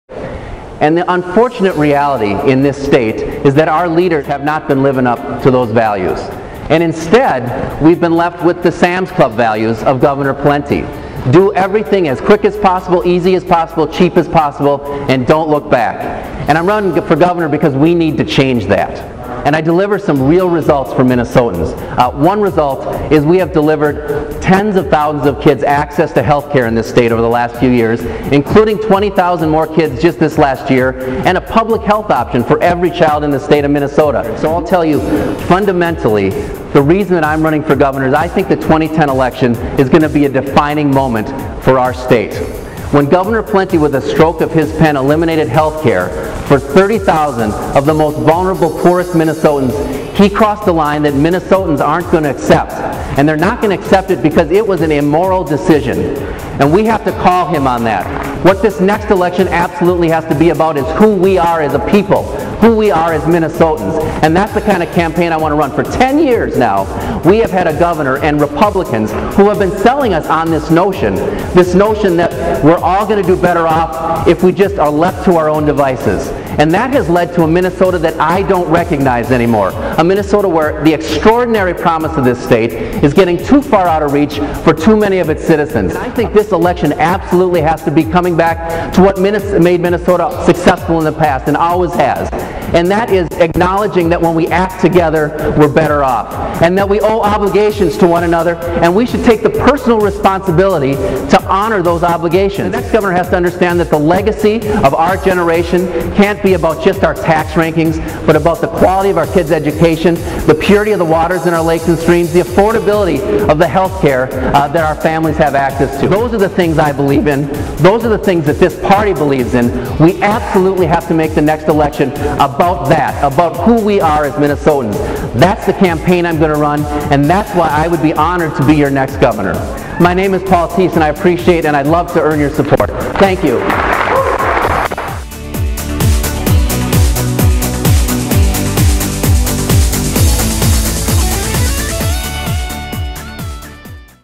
Paul in Hibbing
"This election absolutely has to be about coming back to what made Minnesota successful in the past.  And that is acknowledging that when we act together, we're better off."